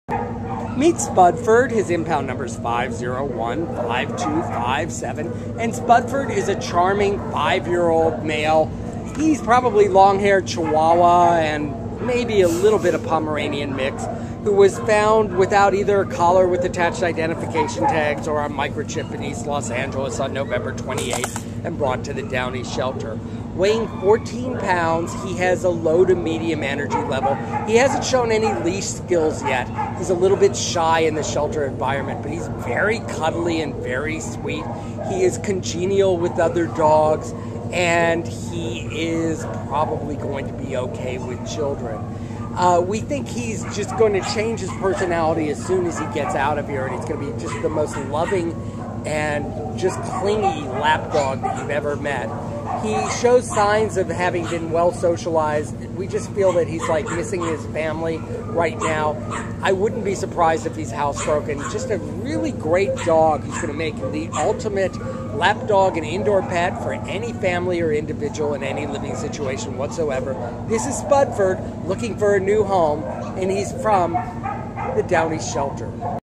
Spudford A5015257
Spudford is a charming five year old black and white male Long Haired Chihuahua/Spaniel mix who was found without either a collar with attached identification tags or a microchip in East Los Angeles on November 28th and brought to the Downey Shelter. Weighing fourteen pounds, Spudford has a low to medium energy level and could use some practice walking on leash.  He is congenial with the dogs he is kenneled with and we think he will do nicely with children. He is a bit timid in the chaotic shelter environment, but you can tell he has a warm and loving disposition. He is quite the snuggler and had to objection whatsoever being held and petted by the volunteers that came over to see him.  Spudford is a loving indoor pet for anyone in any living situation.